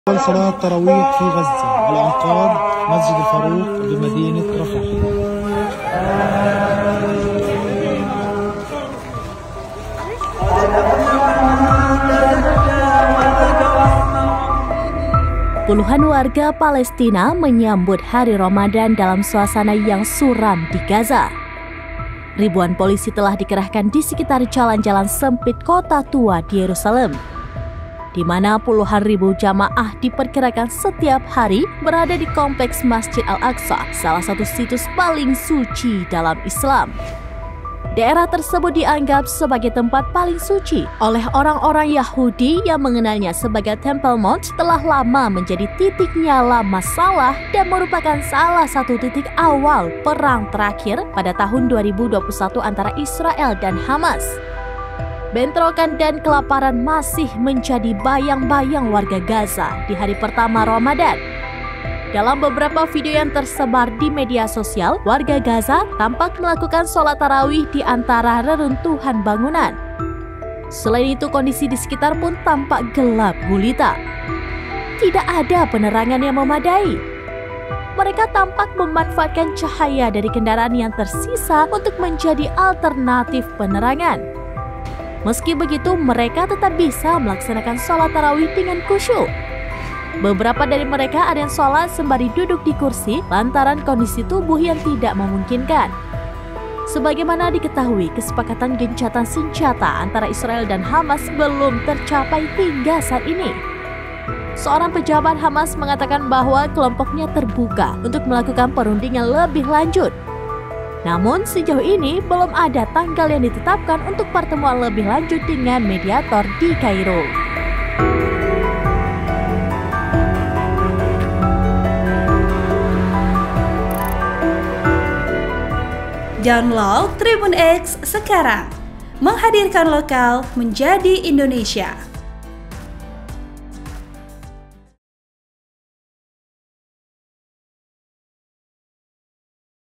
MIRIS! Warga Gaza Salat Tarawih Hari Pertama di Antara RERUNTUHAN Tanpa Penerangan
SERAMBINEWS.COM - Kondisi warga Palestina menyambut bulan Ramadhan dalam suasana yang suram di Gaza.

Ribuan polisi ditempatkan di jalan-jalan sempit Kota Tua di Yerusalem, di mana puluhan ribu jamaah diperkirakan setiap hari berada di kompleks Masjid Al Aqsa, salah satu situs paling suci dalam Islam.

Daerah tersebut dianggap sebagai tempat paling suci oleh orang-orang Yahudi yang mengenalnya sebagai Temple Mount, telah lama menjadi titik nyala masalah dan merupakan salah satu titik awal perang terakhir pada tahun 2021 antara Israel dan Hamas.

Bentrokan dan kelaparan masih menjadi bayang-bayang warga Gaza di hari pertama Ramadhan.

Selain itu, kondisi di sekitar pun tampak gelap gulita.

Tidak ada penerangan yang memadai.

Mereka tampak memanfaatkan cahaya dari kendaraan yang tersisa untuk menjadi alternatif penerangan.

Meski begitu, mereka tetap bisa melaksanakan salat tarawih dengan khusyuk.

Beberapa dari mereka ada yang salat sembari duduk di kursi lantaran kondisi tubuh yang tak memungkinkan.

Sebagaimana diketahui, kesepakatan gencatan senjata antara Israel dan Hamas belum tercapai hingga saat ini.

Seorang pejabat Hamas mengatakan bahwa kelompoknya terbuka untuk melakukan perundingan lebih lanjut, namun sejauh ini belum ada tanggal yang ditetapkan untuk pertemuan lebih lanjut dengan mediator di Kairo.

VO: Syita
Editor: Raja